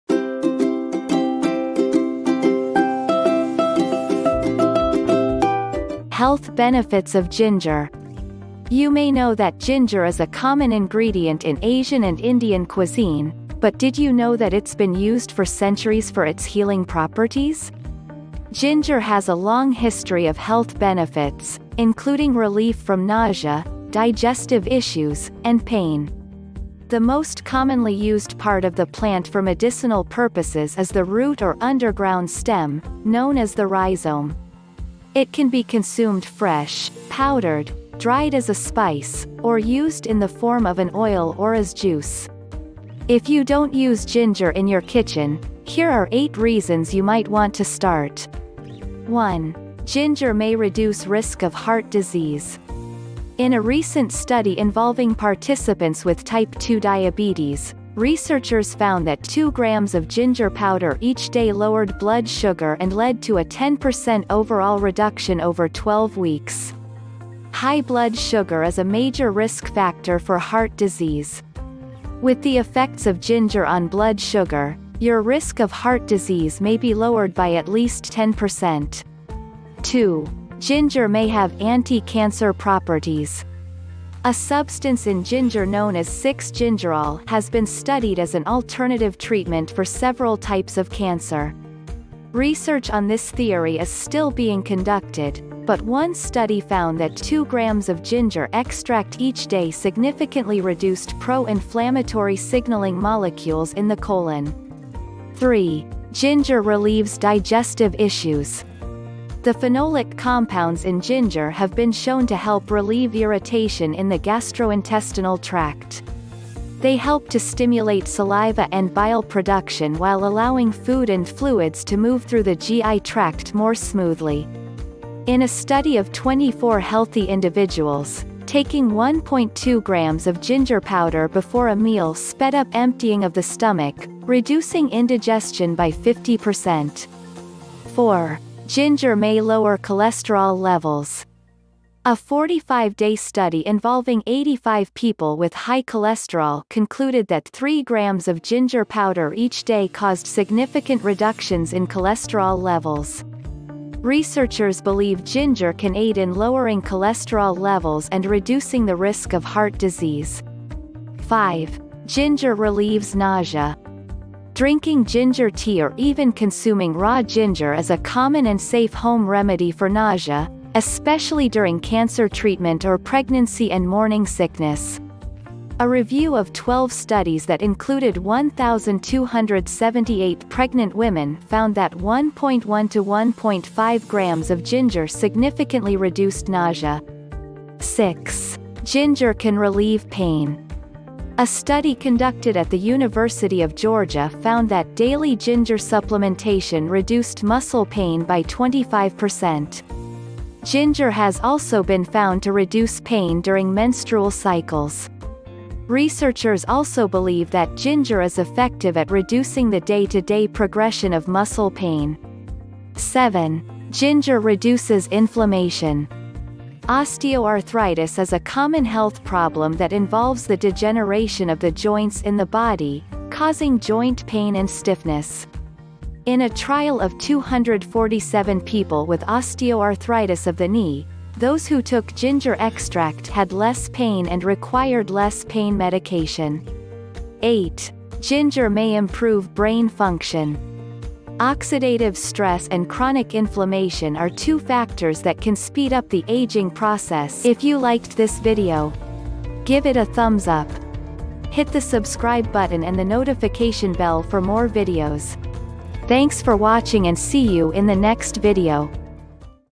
Health Benefits of Ginger | Healthy Lifestyle
Health Benefits of Ginger

You may know that ginger is a common ingredient in Asian and Indian cuisine, but did you know that it’s been used for centuries for its healing properties?

Ginger has a long history of health benefits, including relief from nausea, digestive issues, and pain.

The most commonly used part of the plant for medicinal purposes is the root or underground stem, known as the rhizome. It can be consumed fresh, powdered, dried as a spice, or used in the form of an oil or as juice.

If you don’t use ginger in your kitchen, here are 8 reasons you might want to start!

1. Ginger May Reduce Risk of Heart Disease

In a recent study involving participants with type 2 diabetes, researchers found that 2 grams of ginger powder each day lowered blood sugar and led to a 10% overall reduction over 12 weeks.

High blood sugar is a major risk factor for heart disease.

With the effects of ginger on blood sugar, your risk of heart disease may be lowered by at least 10%.

2. Ginger May Have Anti-Cancer Properties

A substance in ginger known as 6-gingerol has been studied as an alternative treatment for several types of cancer.

Research on this theory is still being conducted, but one study found that 2 grams of ginger extract each day significantly reduced pro-inflammatory signaling molecules in the colon.

3. Ginger Relieves Digestive Issues

The phenolic compounds in ginger have been shown to help relieve irritation in the gastrointestinal tract.

They help to stimulate saliva and bile production while allowing food and fluids to move through the GI tract more smoothly.

In a study of 24 healthy individuals, taking 1.2 grams of ginger powder before a meal sped up emptying of the stomach, reducing indigestion by 50%.

4. Ginger May Lower Cholesterol Levels

A 45-day study involving 85 people with high cholesterol concluded that 3 grams of ginger powder each day caused significant reductions in cholesterol levels.

Researchers believe ginger can aid in lowering cholesterol levels and reducing the risk of heart disease.

5. Ginger Relieves Nausea

Drinking ginger tea or even consuming raw ginger is a common and safe home remedy for nausea, especially during cancer treatment or pregnancy and morning sickness.

A review of 12 studies that included 1,278 pregnant women found that 1.1-1.5 grams of ginger significantly reduced nausea.

6. Ginger Can Relieve Pain

A study conducted at the University of Georgia found that daily ginger supplementation reduced muscle pain by 25%.

Ginger has also been found to reduce pain during menstrual cycles.

Researchers also believe that ginger is effective at reducing the day-to-day progression of muscle pain.

7. Ginger Reduces Inflammation

Osteoarthritis is a common health problem that involves the degeneration of the joints in the body, causing joint pain and stiffness. In a trial of 247 people with osteoarthritis of the knee, those who took ginger extract had less pain and required less pain medication.

8. Ginger May Improve Brain Function

Oxidative stress and chronic inflammation are two factors that can speed up the aging process, and become a key driver of Alzheimer’s disease and other age-related cognitive impairments.

Some studies suggest that the antioxidants and bioactive compounds in ginger can inhibit inflammatory responses that occur in the brain.

In a study of 60 middle-aged women, ginger extract was shown to improve reaction time and working memory, suggesting that ginger supports brain health in multiple ways.

 But as always, If you are in doubt, consult with any knowledgeable health professional before using it. There will be times when you need modern medicines or supplements.